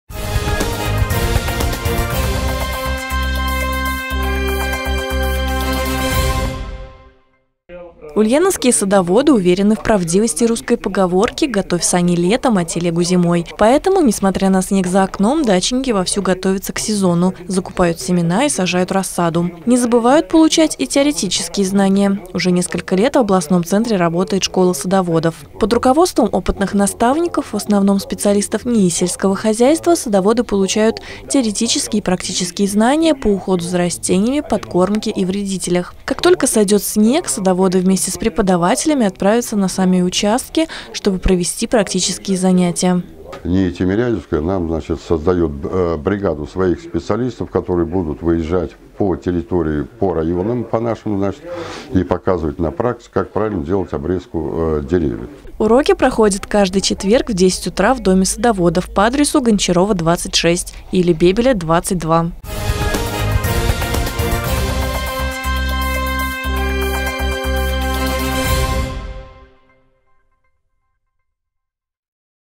Ульяновские садоводы отправились в школу
Ульяновские садоводы уверены в правдивости русской поговорки – готовь сани летом, а телегу зимой. Поэтому, несмотря на снег за окном, дачники вовсю готовятся к сезону – закупают семена, сажают рассаду. Не забывают получать и теоретические знания. Уже несколько лет в областном центре работает школа садоводов.